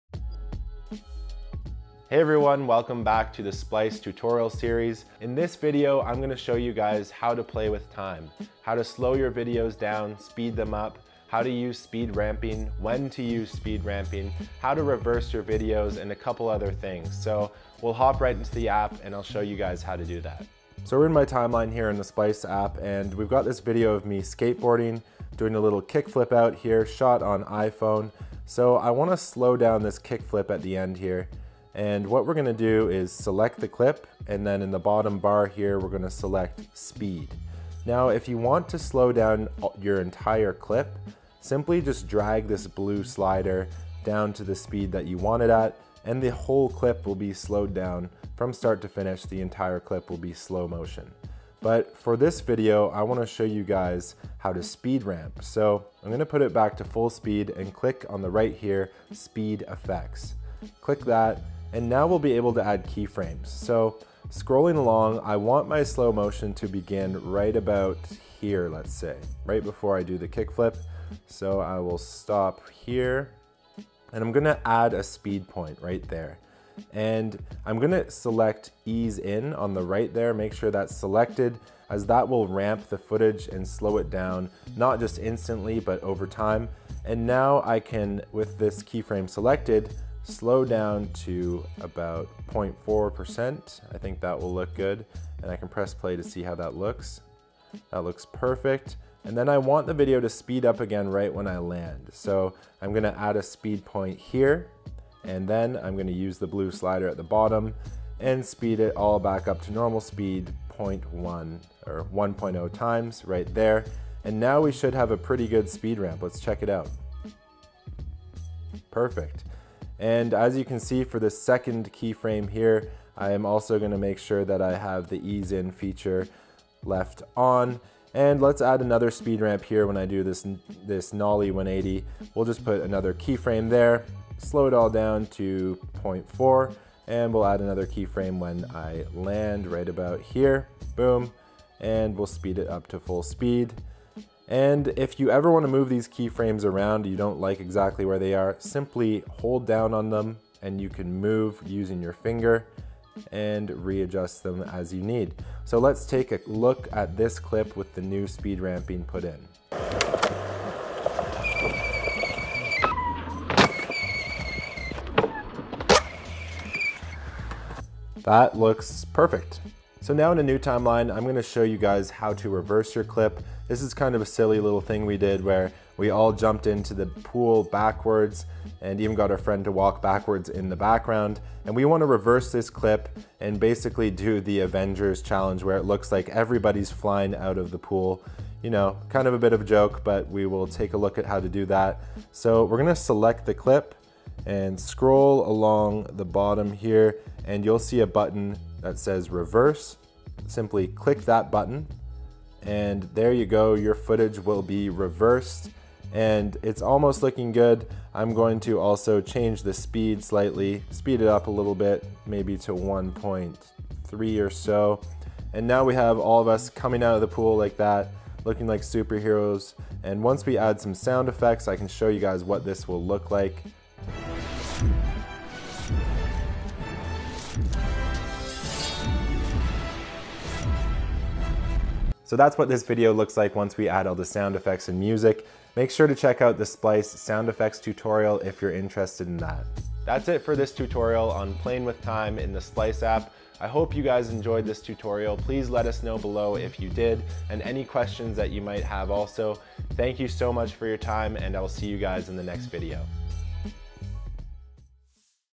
Play with Videos Speed - Slow motion and Speed Ramping
Episode 4. 🎬
Become the master of time! Double-checking. Yep, we got that right! We'll let Ryan tell you all about that!
Speed up, slow down, and reverse your videos in a tap! Oh, and not to forget the speed ramp—for that little extra dramatic and creative touch.
Get ready to play with time!

Video Sections:
0:00 Intro
0:20 How to create slow-motion video or sections
0:51 Use keyframes to create speed ramps
2:49 Reverse you clips for impressive effects
3:59 Outro

"The Best Video Editor for iPhone"
- Lifehacker